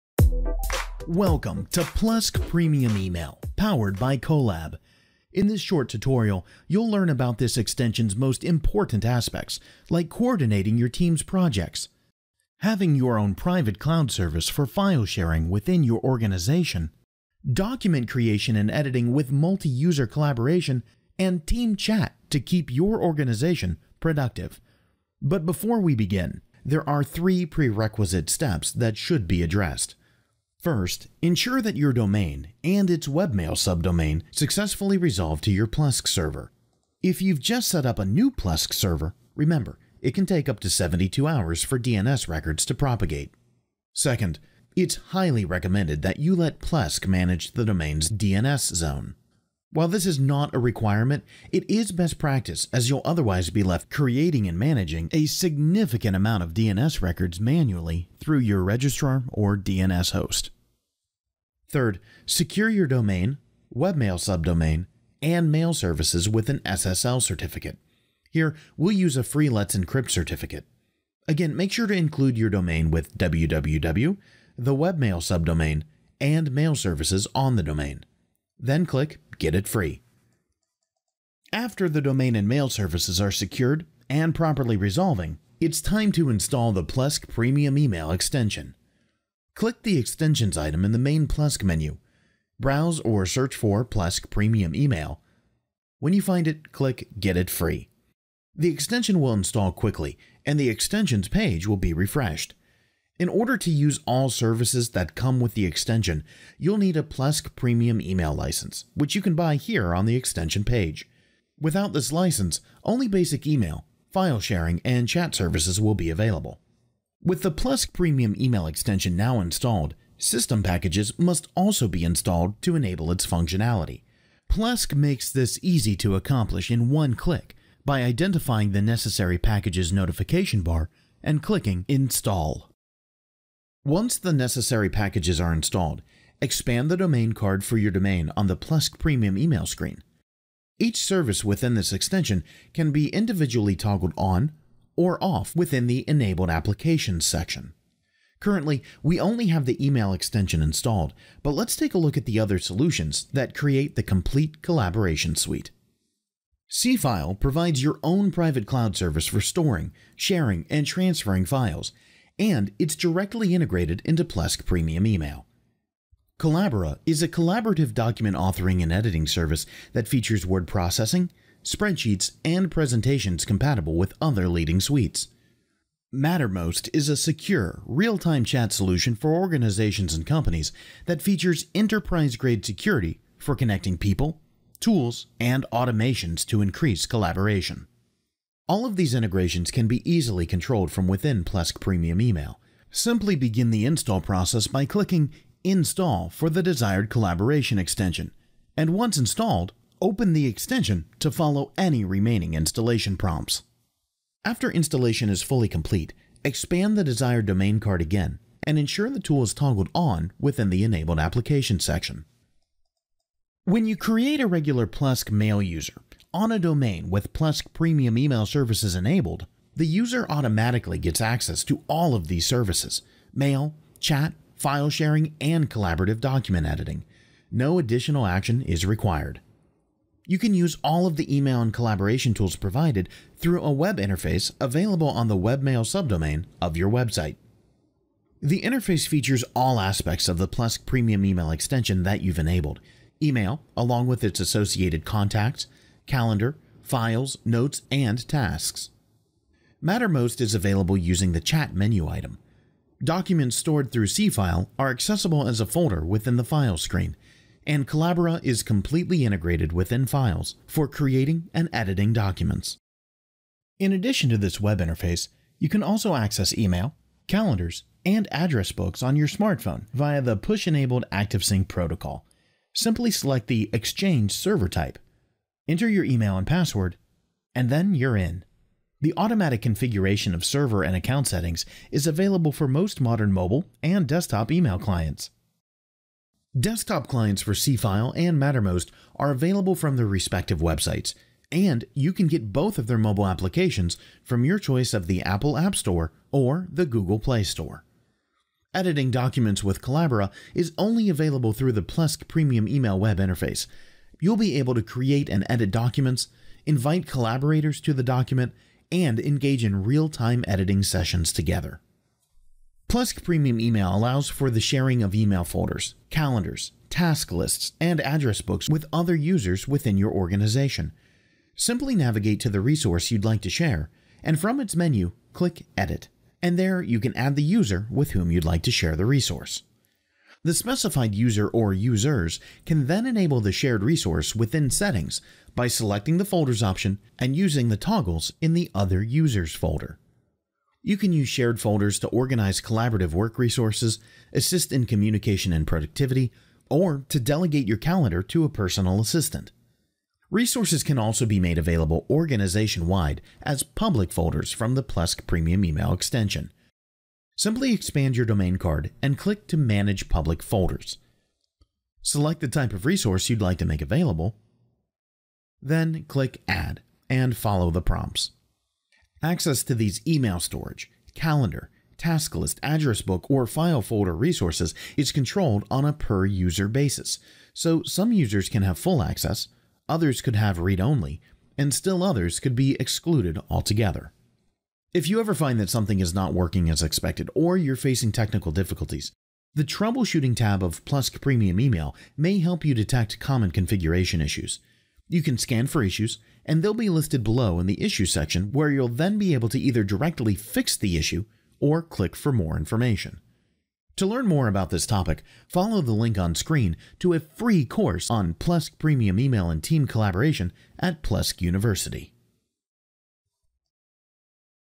Plesk Premium Email Tutorial
Practical Self-Hosted Online Office - easily send and receive emails with Plesk Premium Email, powered by Kolab. Enjoy the responsive browser-based interface on both desktop and mobile devices. Manage e-mail, calendars, contacts, files, tasks, and notes. Scale as needed – start from one mailbox and add up to 500. Stay safe with a high level of privacy and control. All data and components stay on your personal Plesk server.